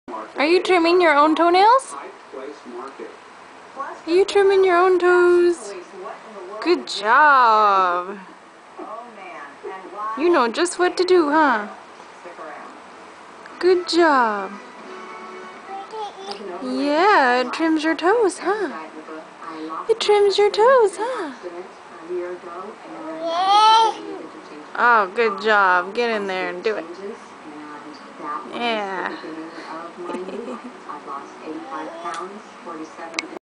Pretending to trim her own toe nails
1 year 2 weeks old